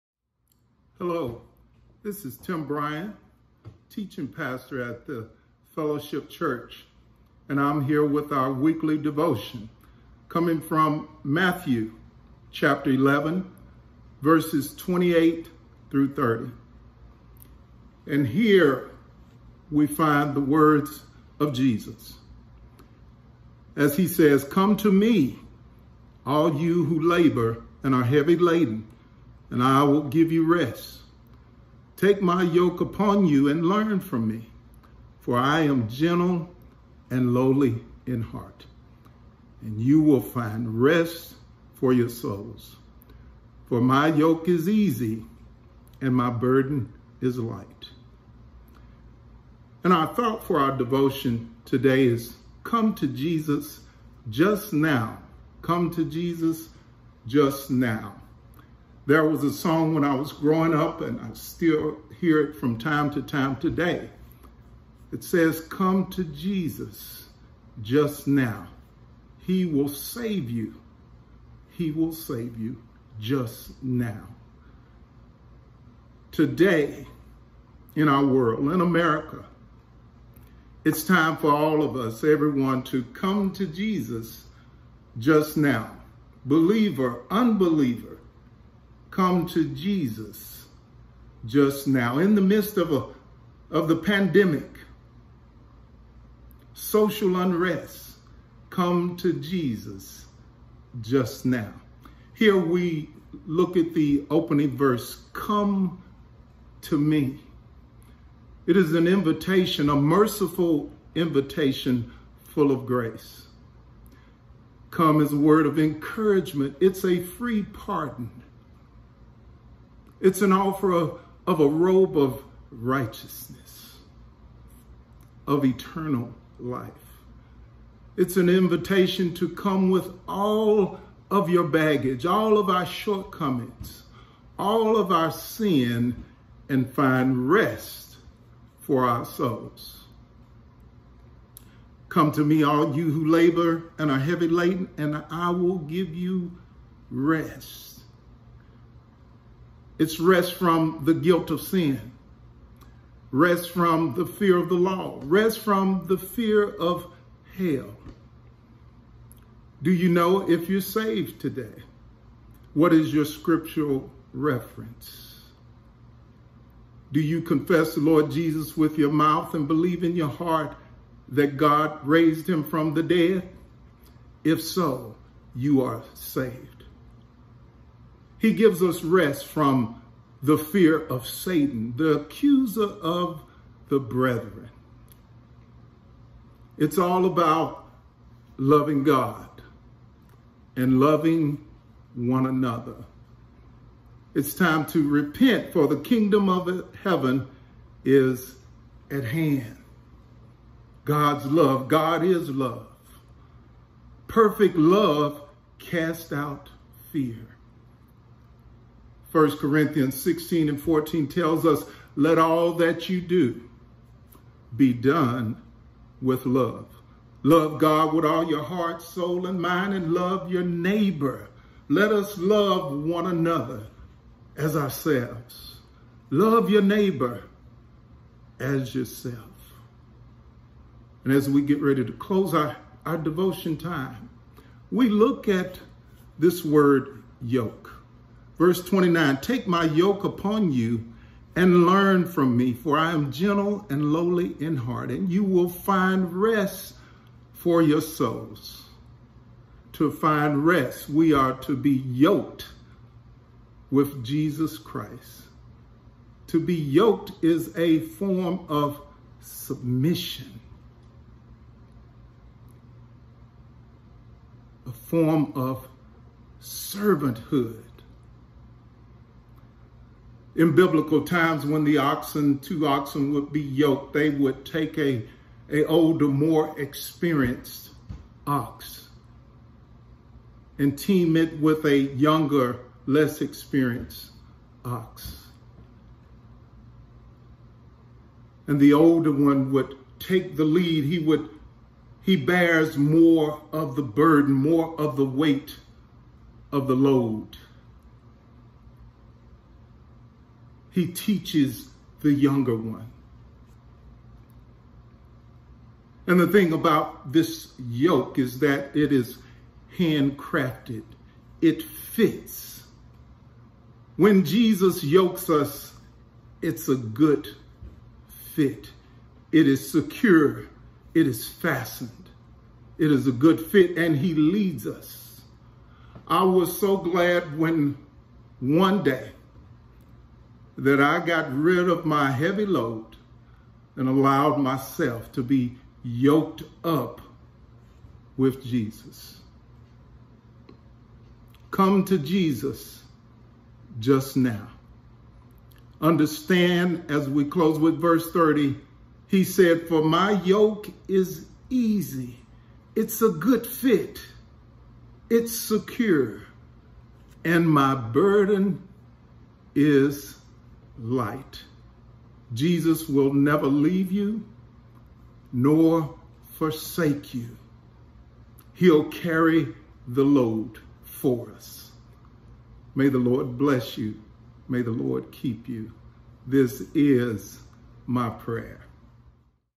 Fellowship Mt Juliet- Friday devotional 8/21/20
FMJ Friday devotional for August 21, 2020 with teaching pastor Tim Bryant.